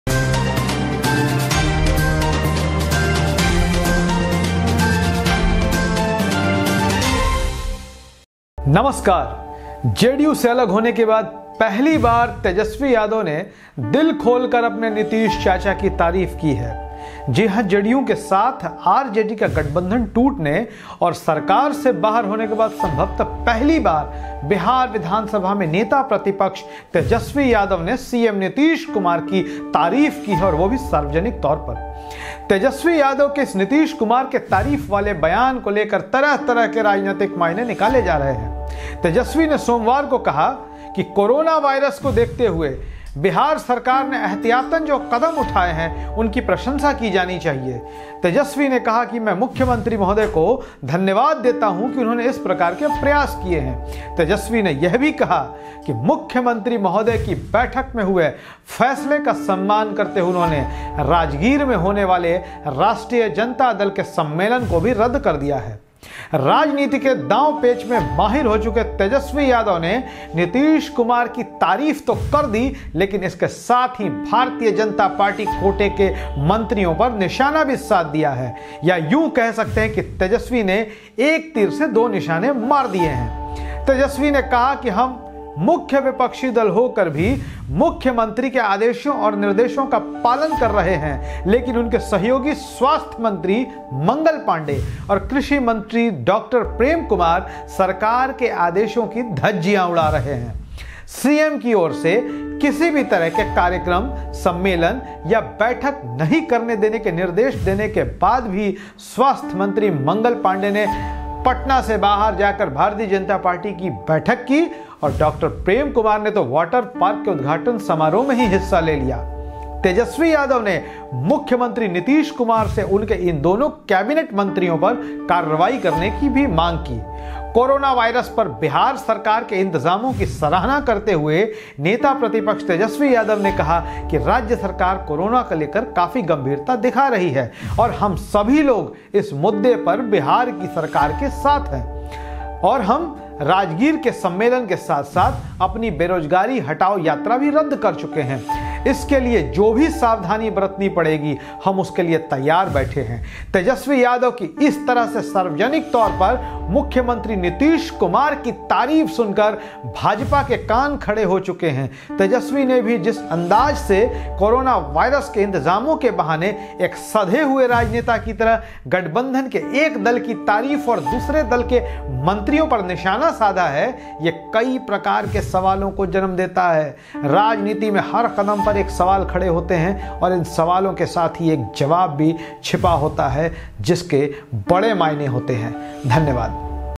जेडीयू से अलग होने के बाद पहली बार तेजस्वी ने दिल खोल कर की नीतीश चाचा की तारीफ
Everyday new alliances are formed in politics. Old alliances break up. Equations are formed. The equations deteriorate. Nitishkumar praise of Tejashwiyadav is not just like this. There are many secrets hidden in it.